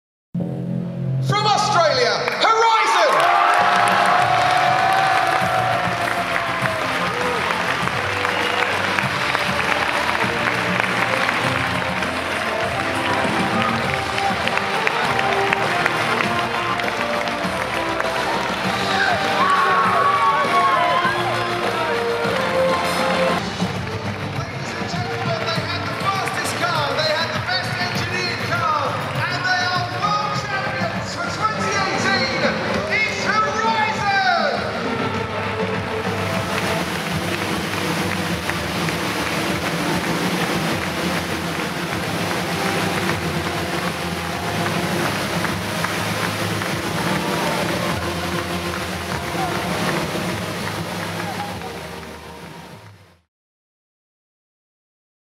Horizon - 2018 F1 in Schools World Champions
The moment our ambitions became reality. One year ago today, we became World Champions at the 2018 F1 in Schools World Finals in Singapore.